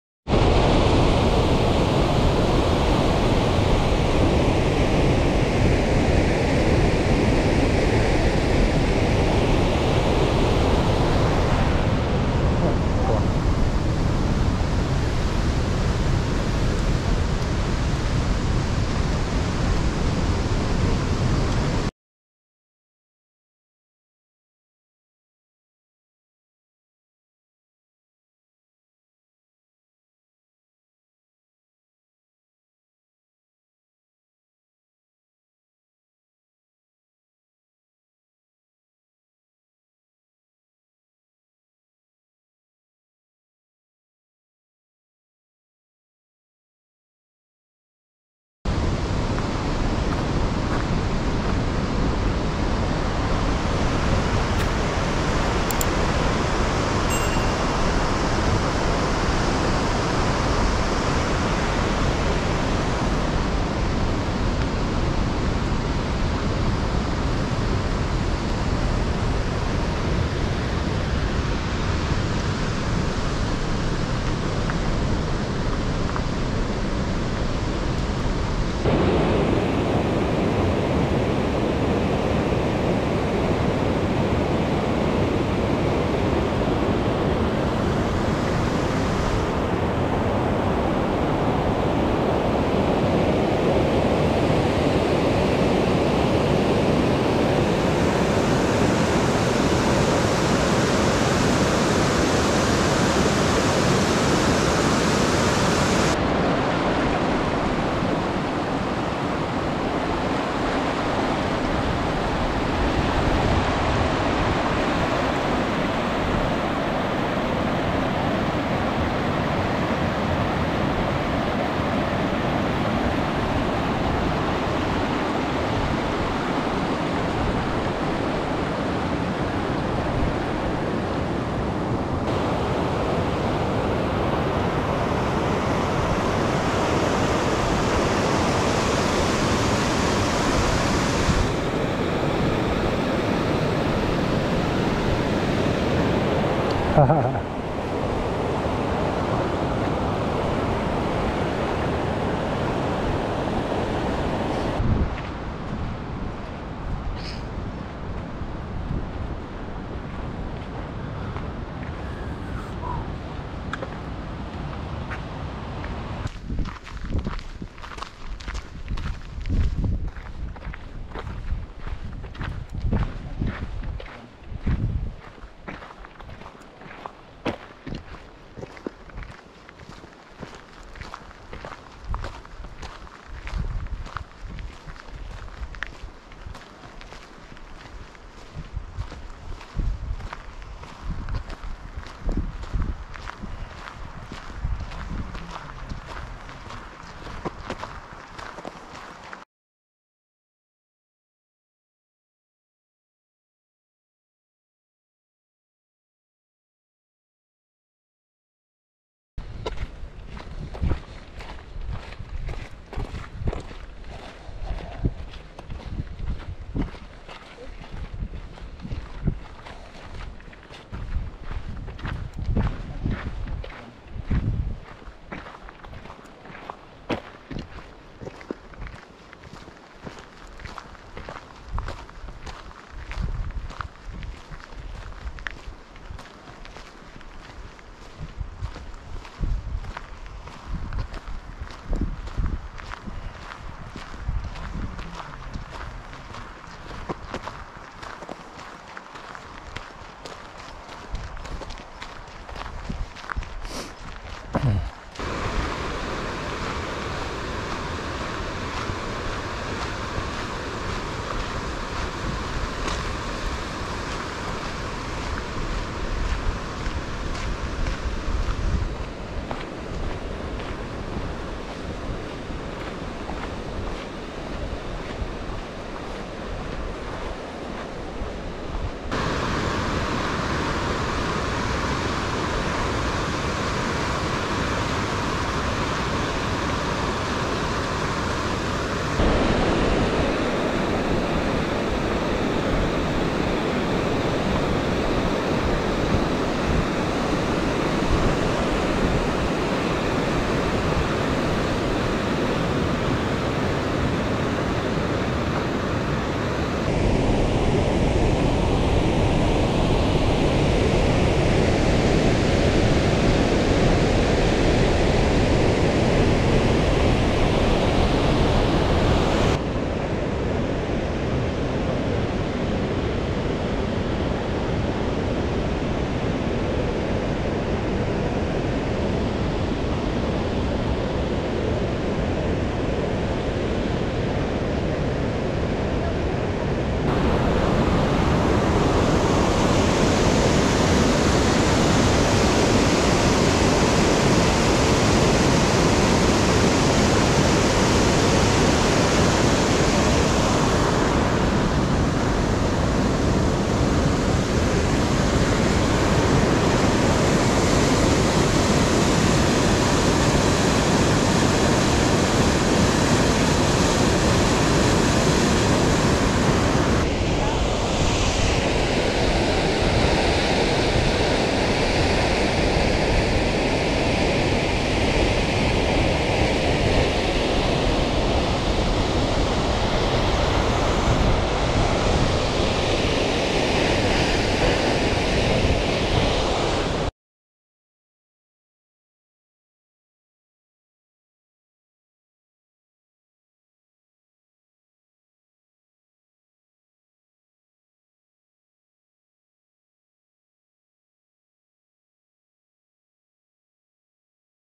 # 12 Waterfall and Alm (Krimml)
A short video of the hike up the mountains next to the Krimml waterfall in Austria. Beautiful views, thunderous water and a nice refreshing beer on the alm.